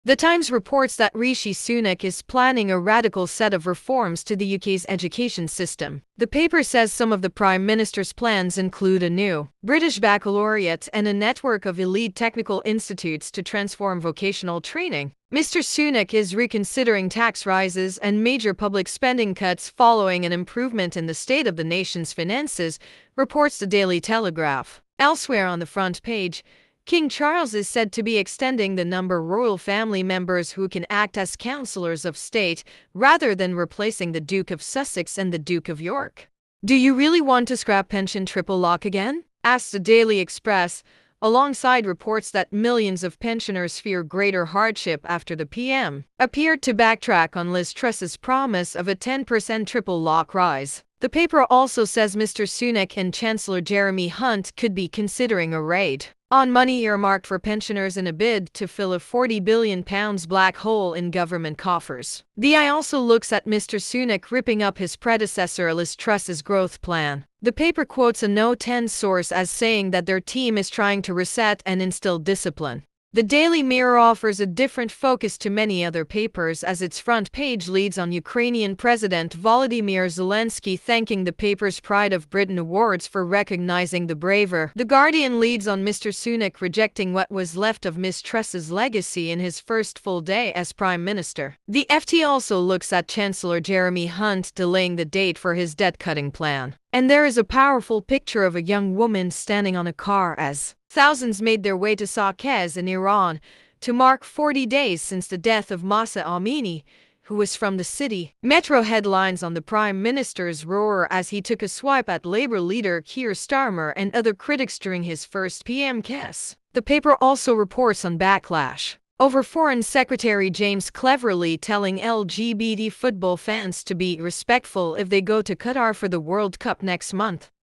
Newspaper headlines: 'Rishi's roar' and 'Truss legacy rejected'
You can join our telegram channel for the latest news.

In this video, we'll take a look at some of the headlines from the newspapers this week.

First, we'll take a look at the headline for "Rishi's roar" which talks about how Rishabh Pant's century has rocked the Indian domestic scene.

Then, we'll take a look at the headline for "Truss legacy rejected" which discusses how the Australian selectors have rejected the Test captaincy for Steve Smith.